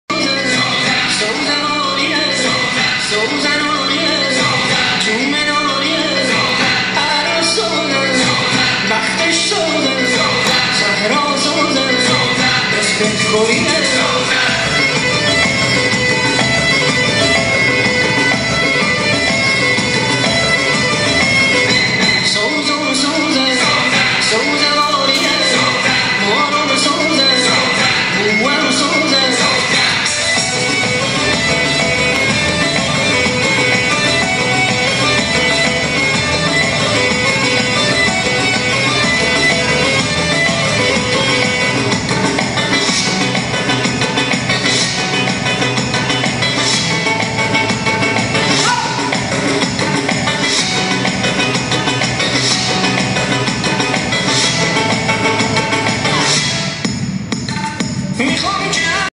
Изабелла Павлюк Фольклор Неегипетский Бандари
Изабелла Павлюк « Фольклор Неегипетский. Бандари. Стиль,костюм,лексика,состояние,особенности исполнения. На примере постановки»